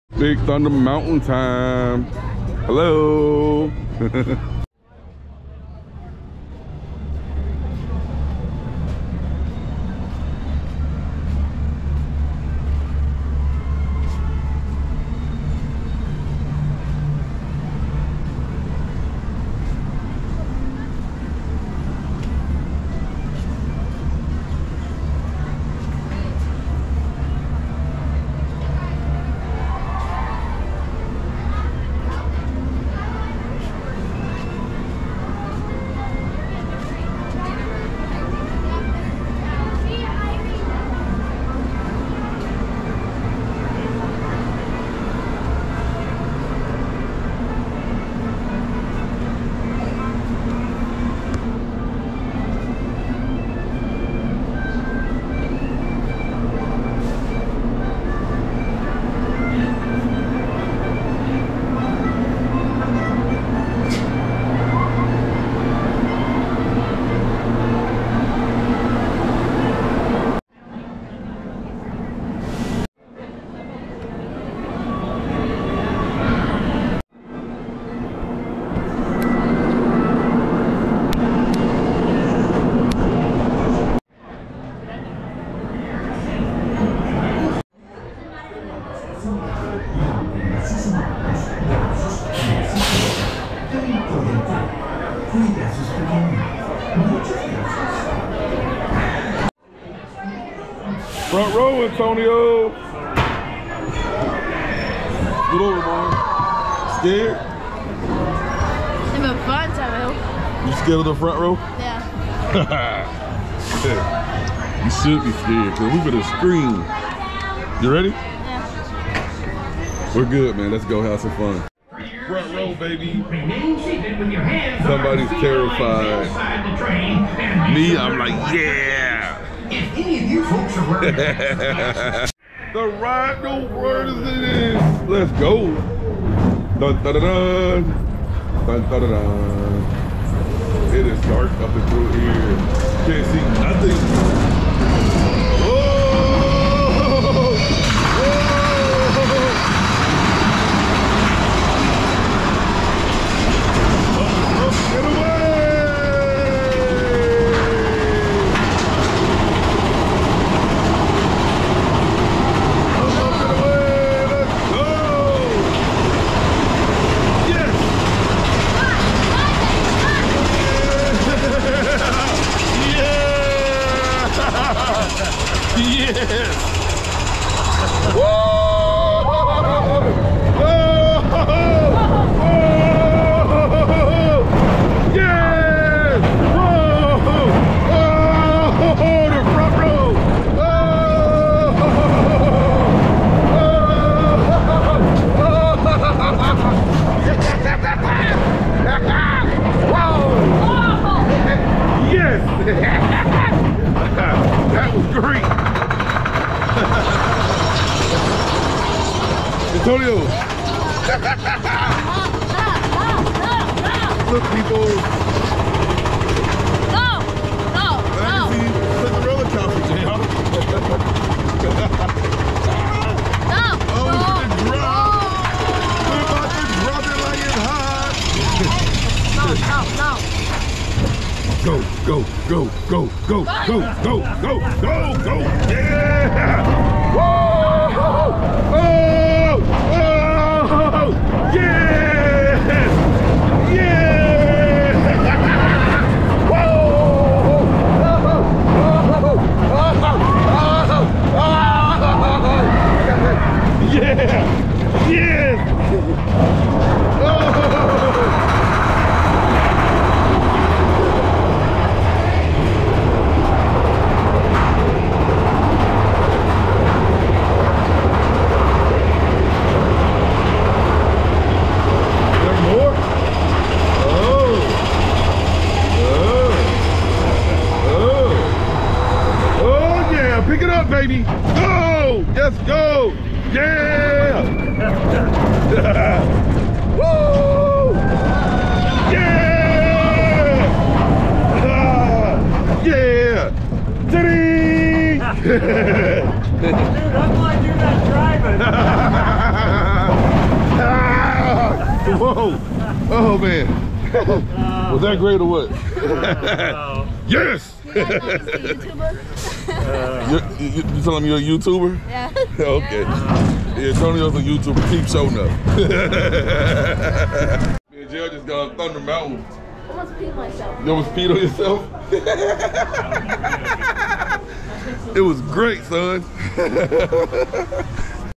Big Thunder Mountain Magic Kingdom COMPLETE Ride Experience in 5k| Walt Disney World
Big Thunder Mountain  my favorite  ride at magic kingdom come in enjoy as I cruise on it before  hurricane  Ian hits Florida...